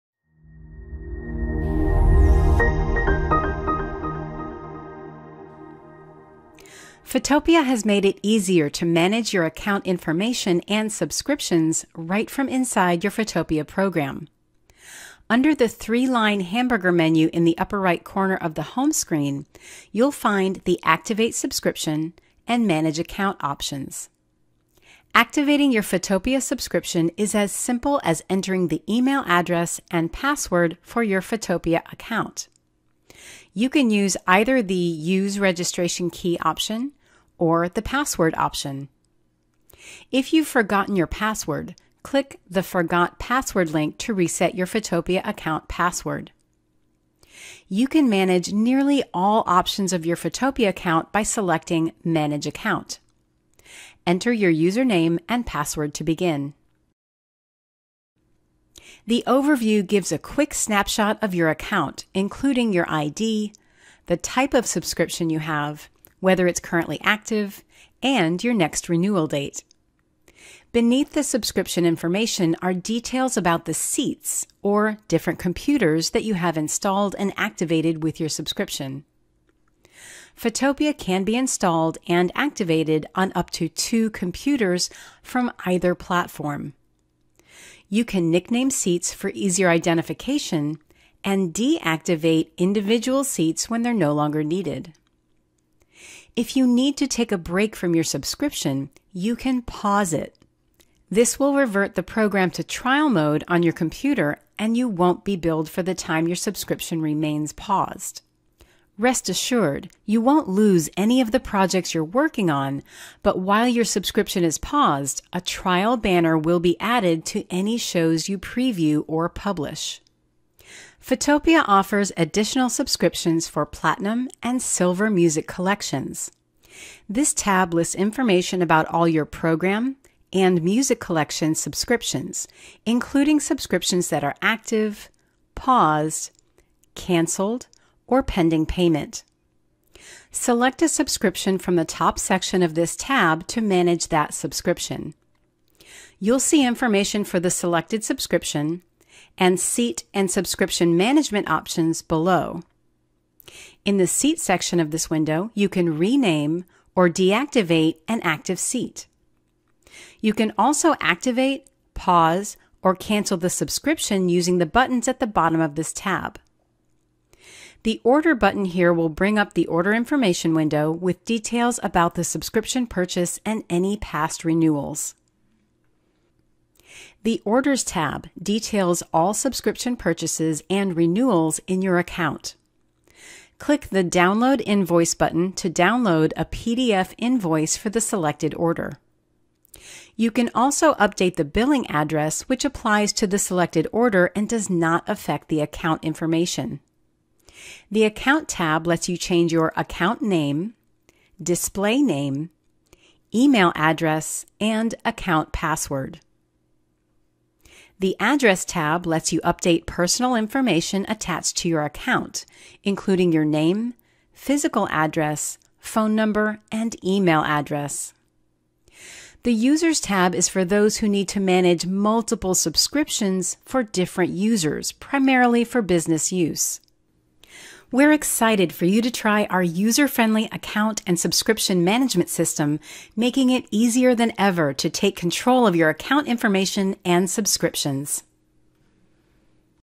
Photopia Account Management
The latest update of Photopia Director and Creator lets you manage your subscription right from the program. Learn more in this tutorial video.